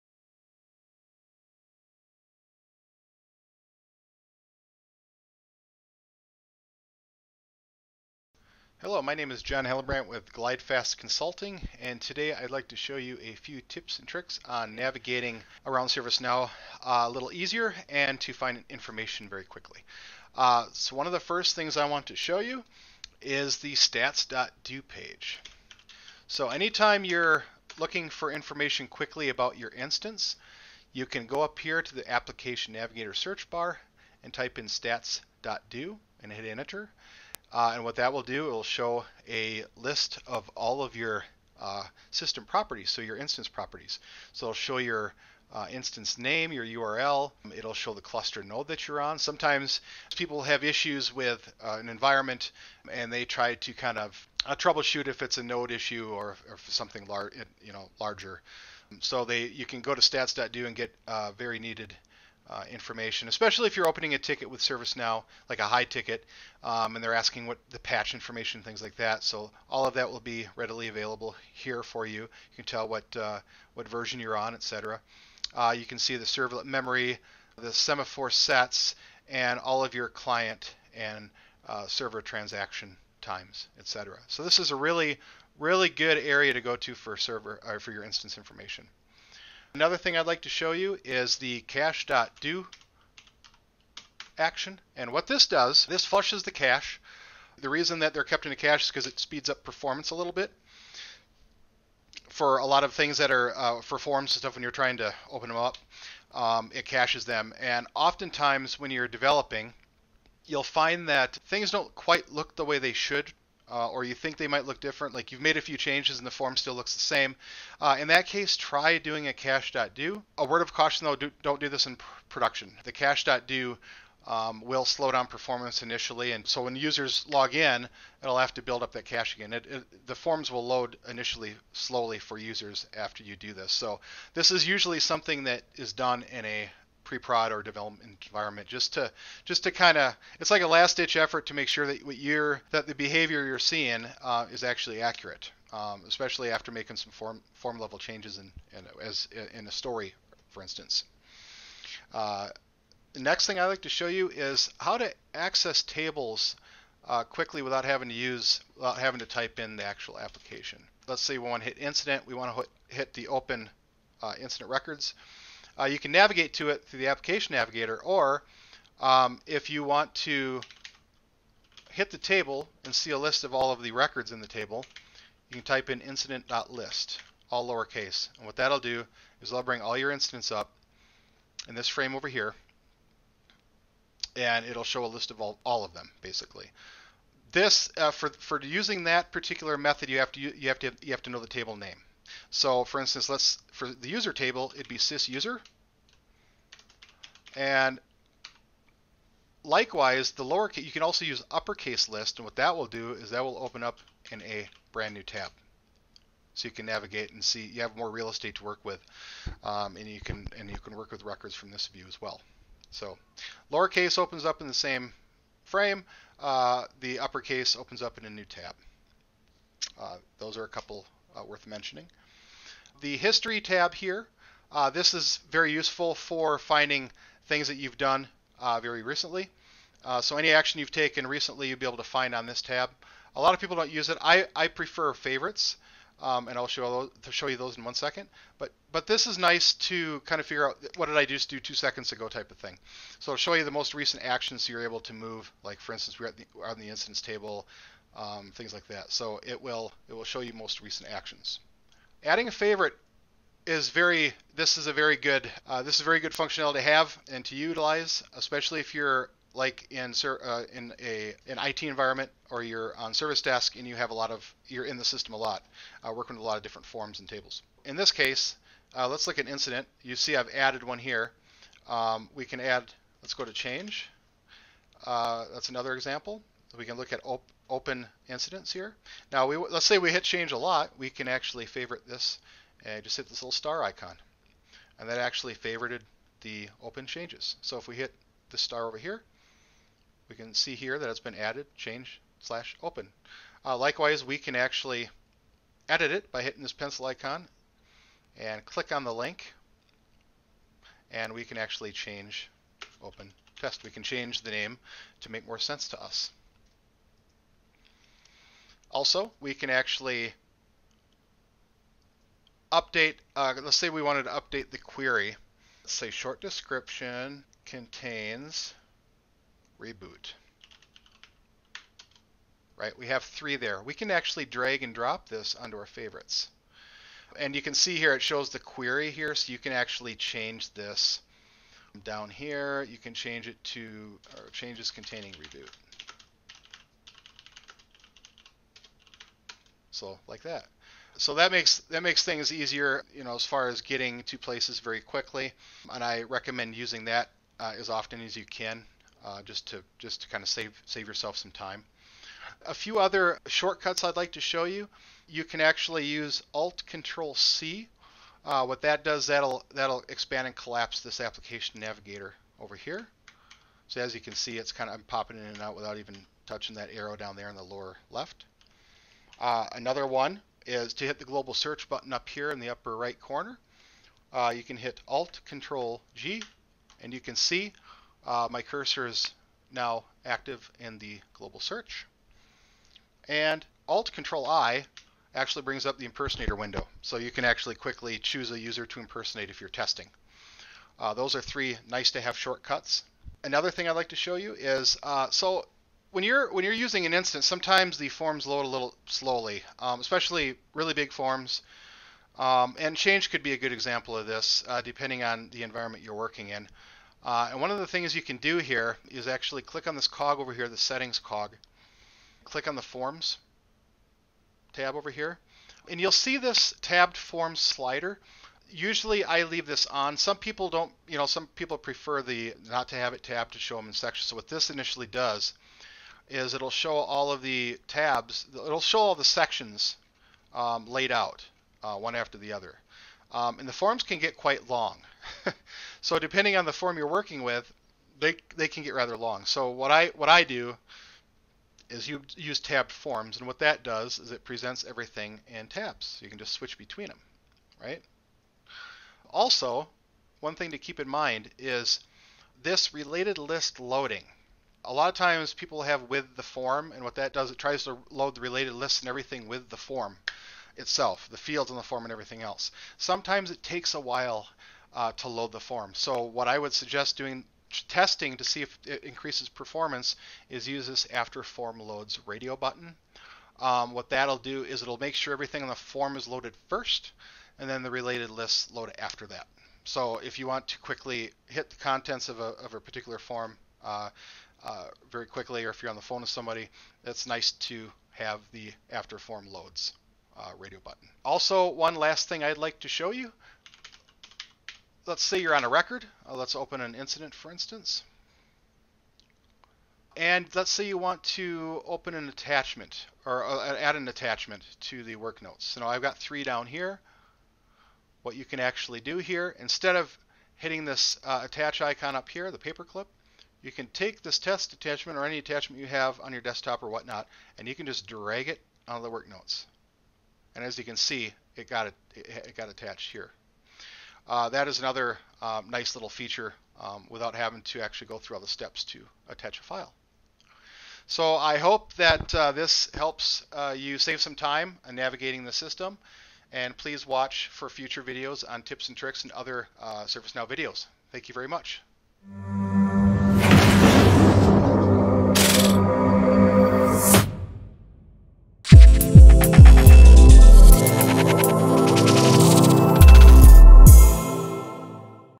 ServiceNow Tips & Tricks 3 for Navigation | Share the Wealth
Learn tips and tricks for navigation in ServiceNow during this session of Share the Wealth.

The application navigator, or left navigation bar, provides access to all applications and the modules they contain, enabling users to quickly find information and services.